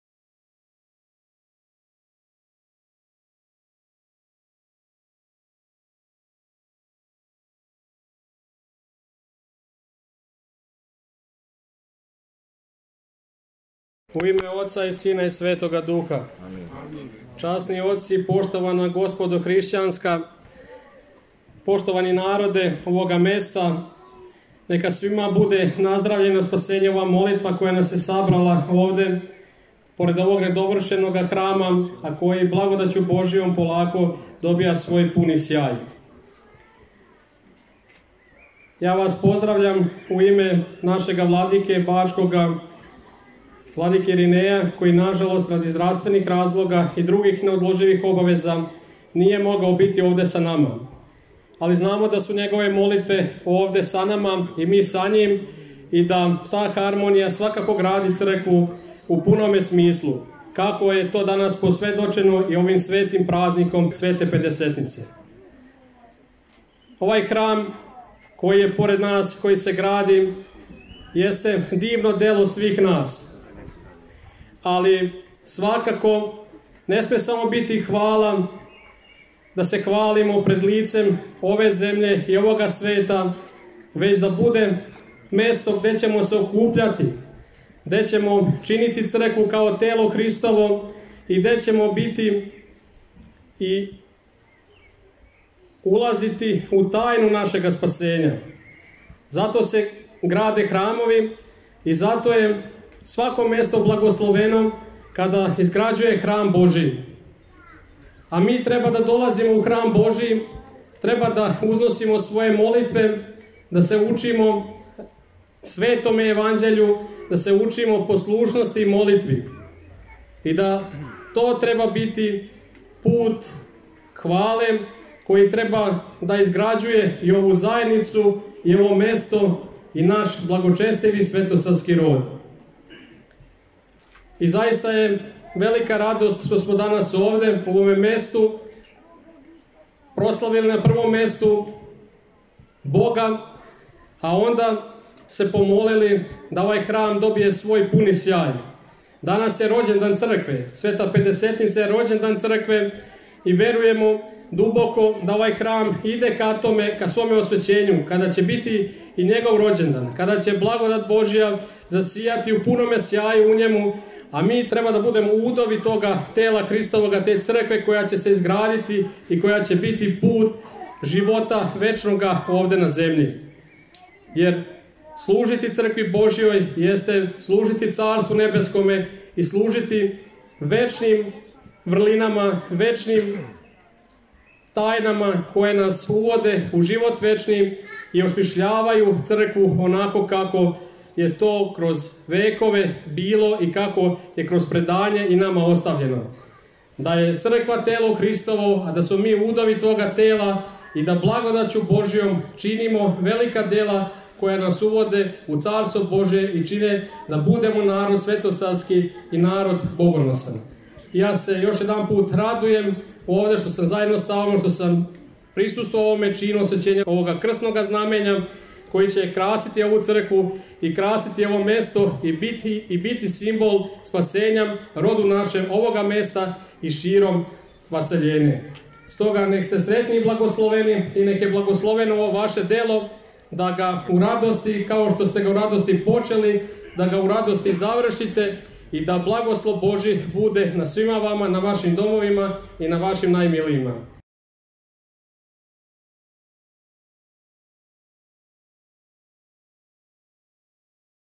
Епископ осечкопољски и барањски г. Херувим, Обраћање у Новом Жеднику, 4. јун 2023.
Пратите садржаје Информативне службе Епархије бачке на интернет страници: eparhijabacka.info

Као и на друштвеним мрежама:
Јутјуб @eparhija_backa